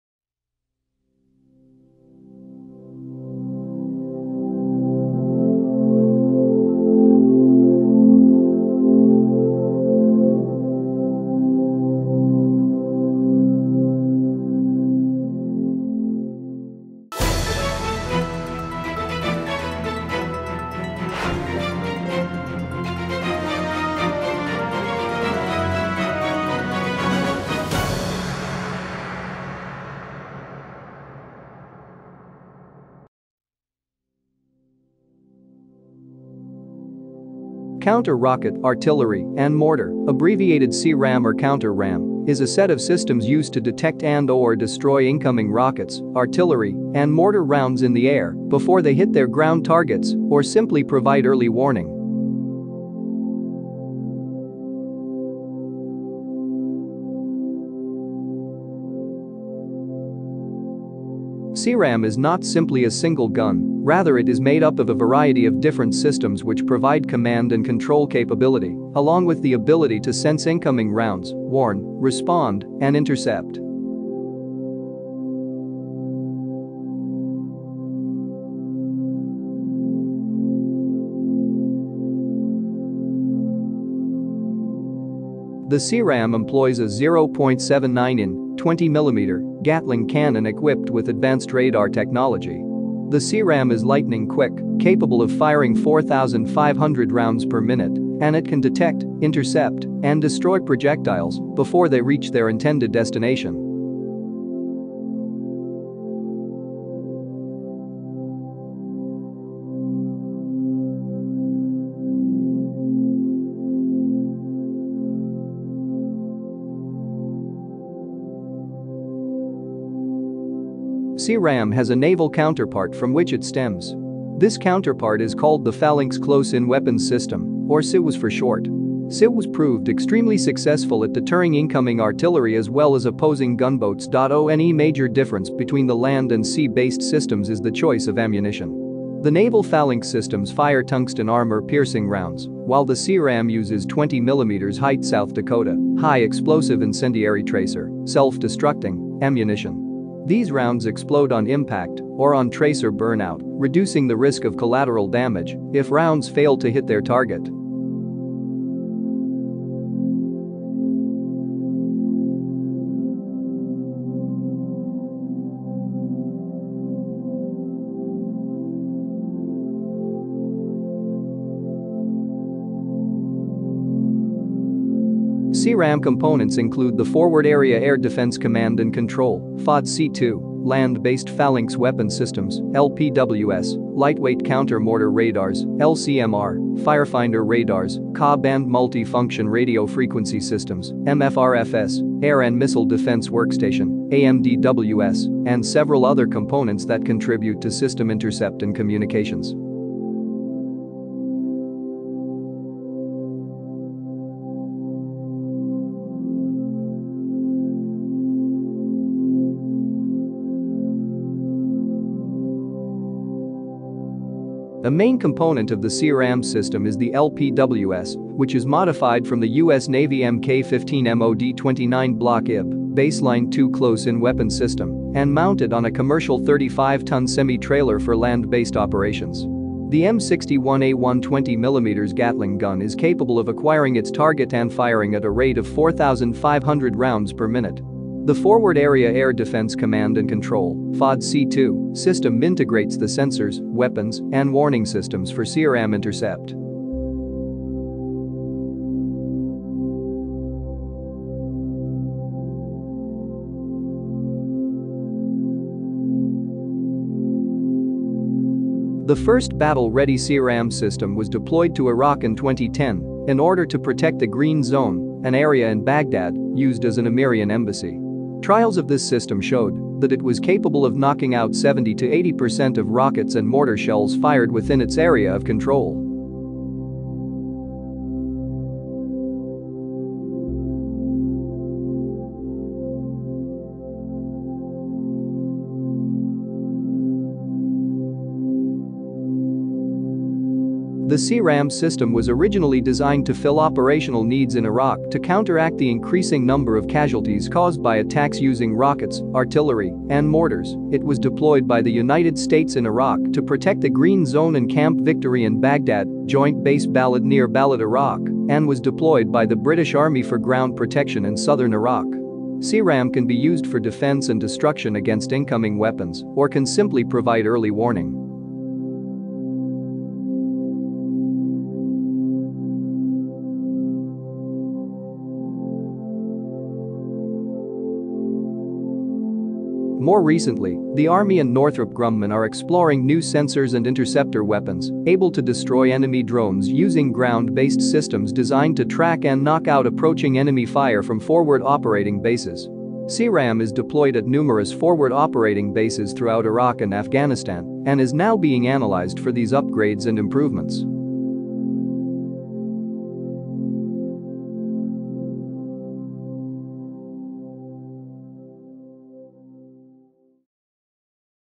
Get to know C-RAM-Anti-rocket defense weapon belonging to the United States military
Counter rocket, artillery, and mortar, abbreviated C-RAM or counter-RAM, is a set of systems used to detect and/or destroy incoming rockets, artillery, and mortar rounds in the air before they hit their ground targets, or simply provide early warning.

C-RAM is not simply a single gun, rather it is made up of a variety of different systems which provide command and control capability, along with the ability to sense incoming rounds, warn, respond, and intercept.